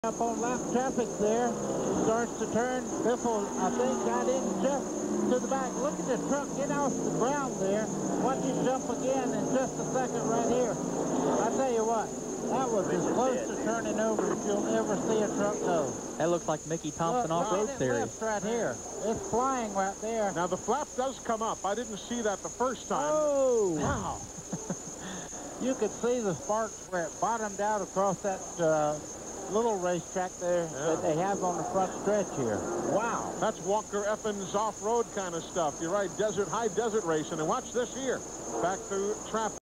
1998 Pronto Auto Parts - Jack Sprague Near Flip
1998 NASCAR Craftsman Truck Series @ Texas